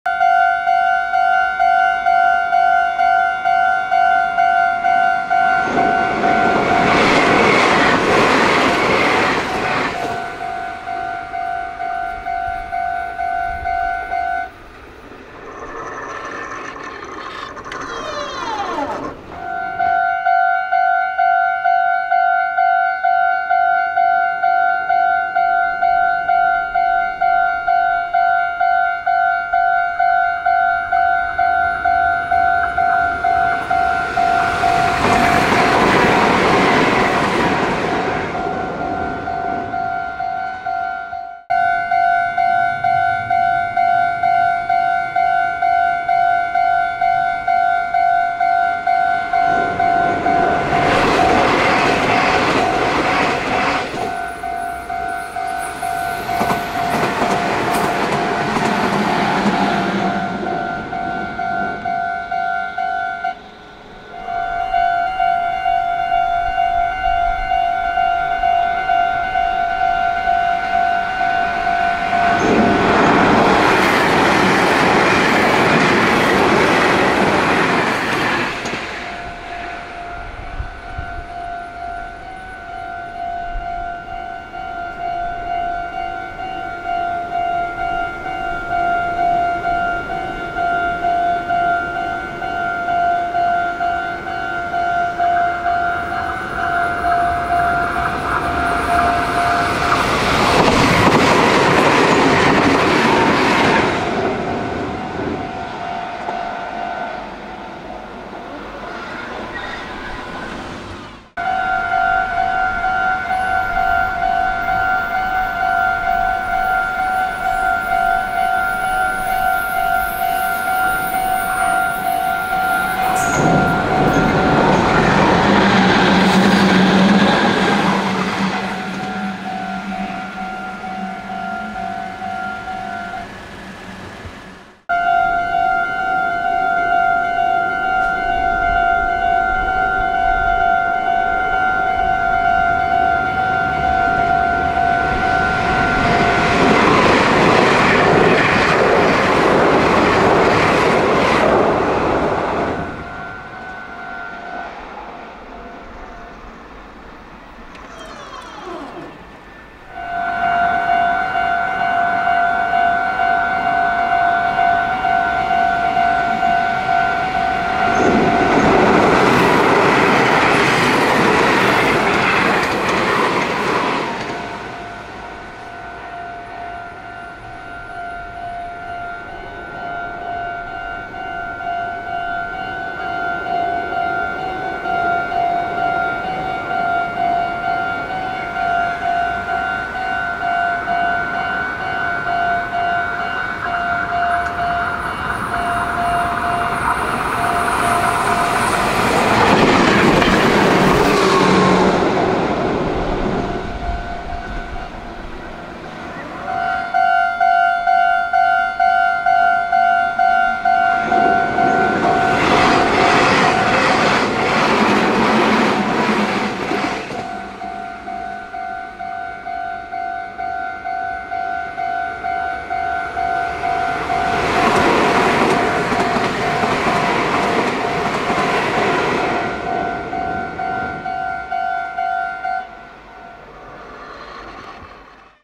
【鉄分チャージ】高速カーブを駆け上がる名鉄電車が迫力ありすぎ⁉
【撮影日】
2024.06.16
【撮影場所】
名鉄常滑線 神宮前-豊田本町
【撮影機材】
SONY FDR-AX60 又は iPhone14
【音楽】